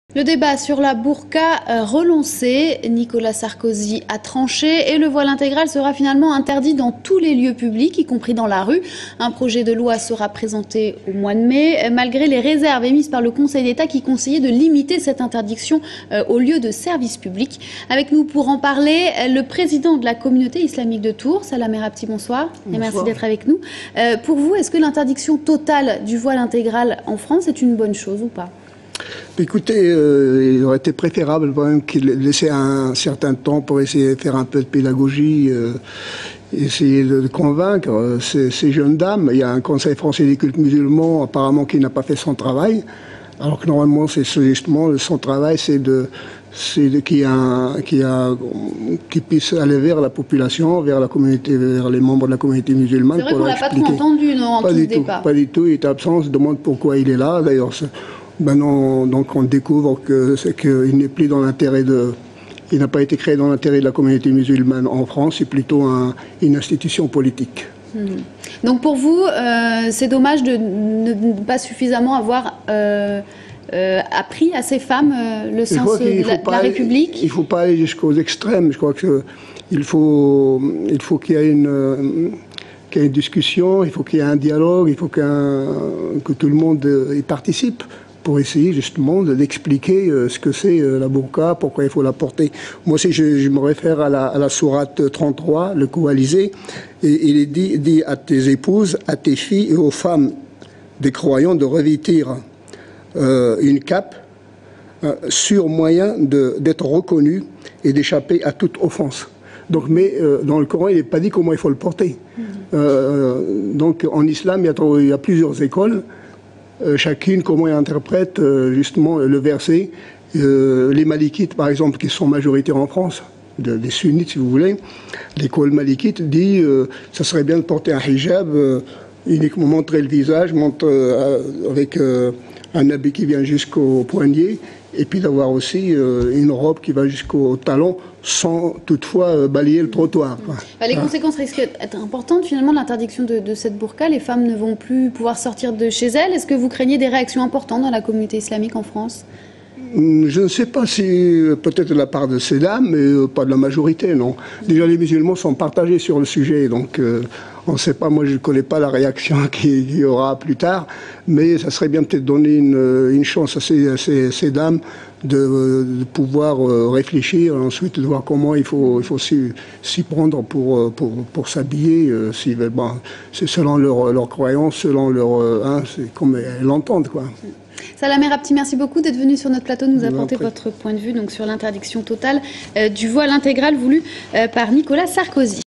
Le débat sur la Burqa relancé
Entretien avec Salah Merabti, Président de la communauté islamique. (DROITS RESERVES . Pour toute exploitation sur TV, web, mobile, aller